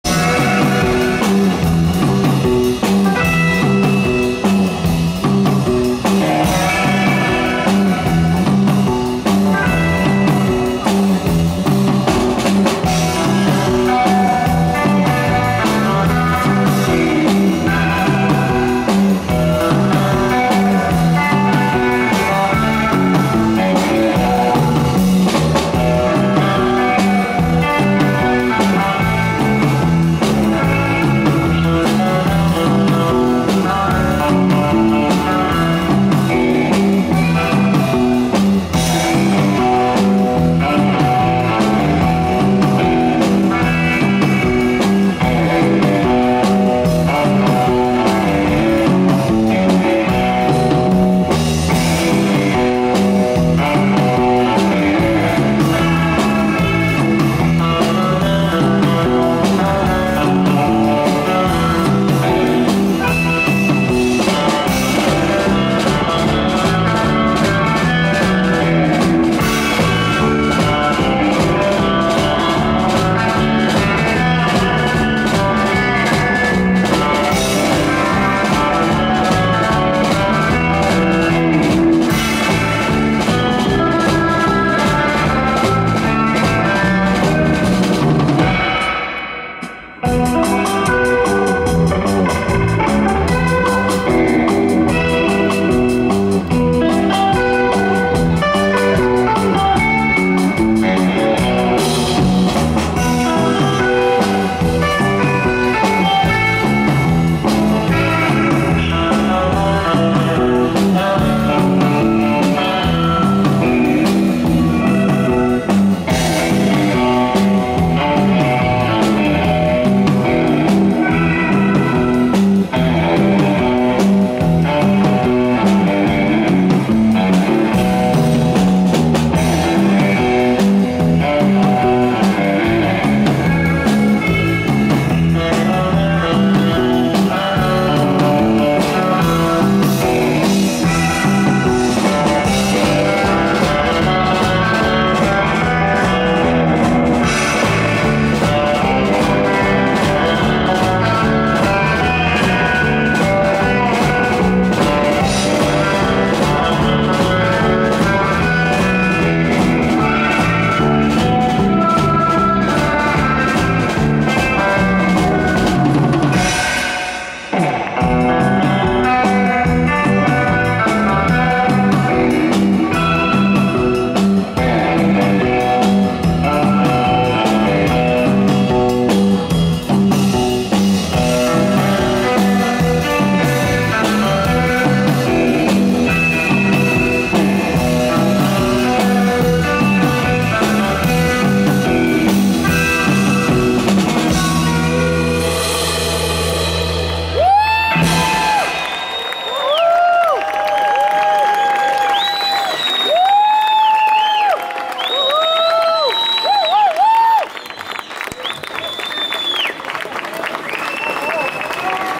MESSER CHUPS - (live Baldaya Fest 23)
Os Messer Chups ao vivo em 16 de setembro de 2023 no Baldaya Vintage Fest que ocorreu no Palácio Baldaya em Benfica.